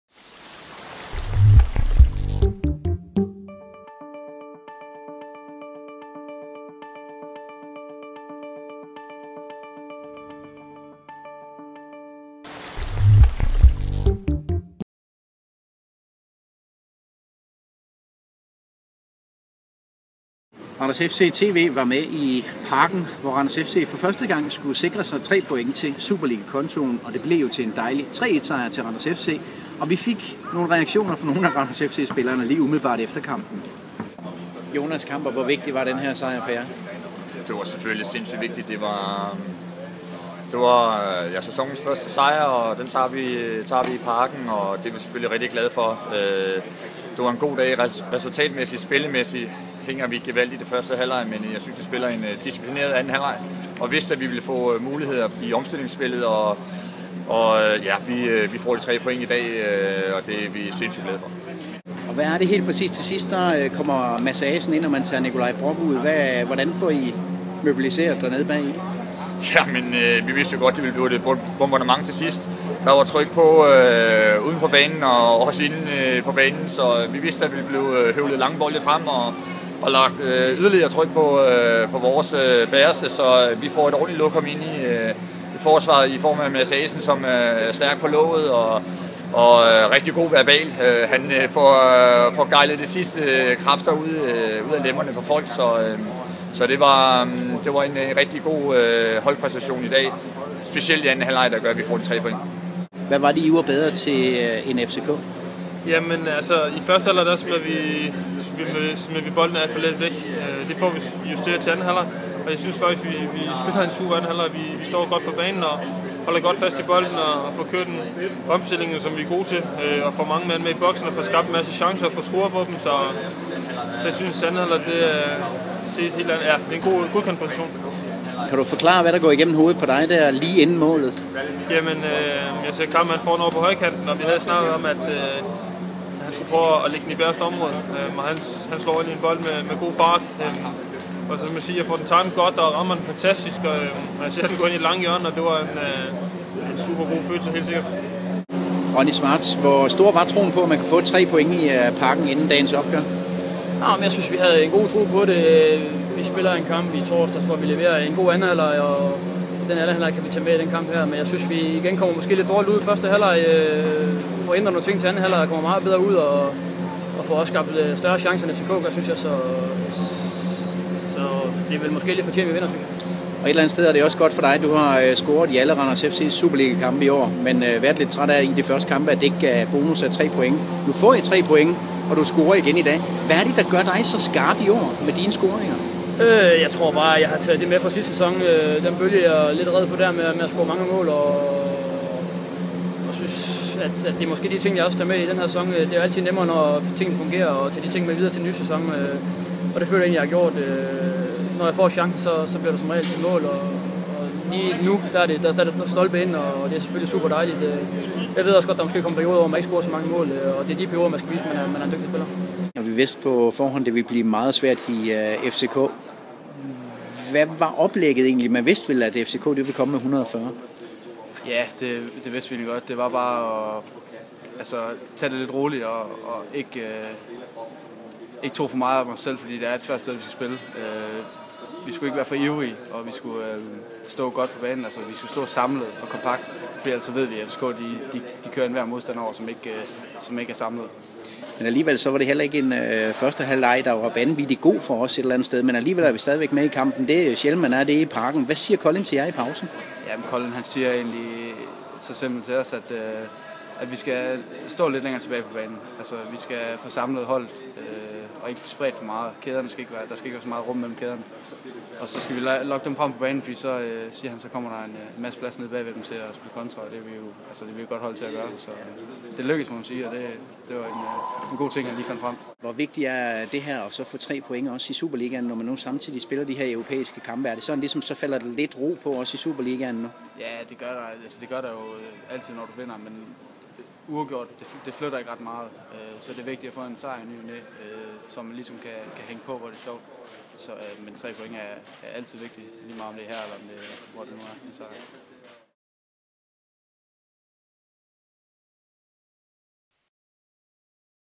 Randers FC TV nedtakt fra Parken
Vi fik en snak med Jonas Kamper, Jonas Borring, Ronnie Schwartz og Mads Fenger efter de 3 point i Parken.